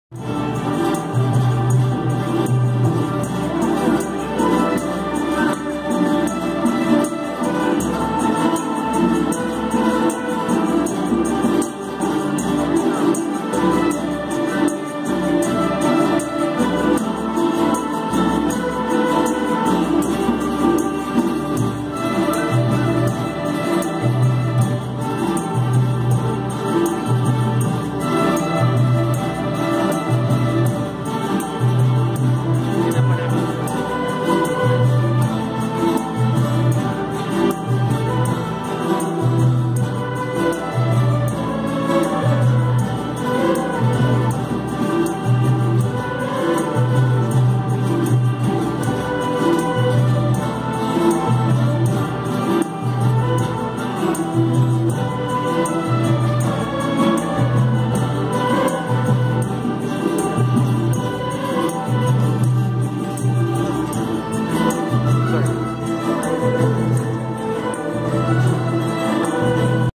folk nepali - orchestra
A clip of my school students.. Their performance during parent's day .